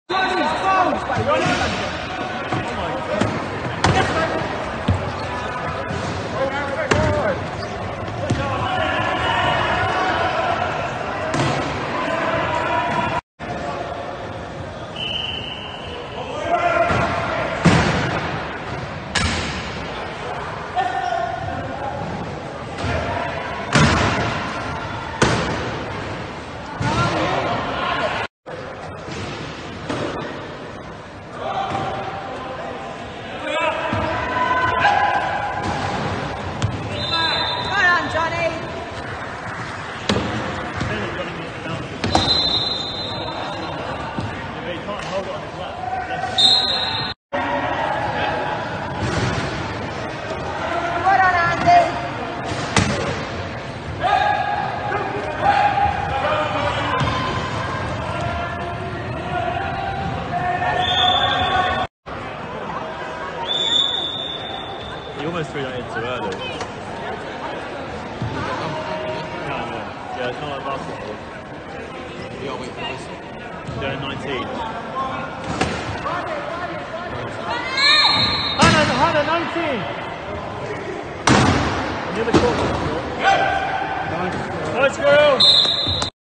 My First Wheelchair Rugby Game in 2019
This is a clip of my very first wheelchair rugby game, played at Stoke Mandeville Stadium in 2019.
there is no other sport like this and as you will soon find out for yourself I am a massive fan and I enjoy it.

Let's inspire more wheelchair users of any kind to come out and enjoy the sport.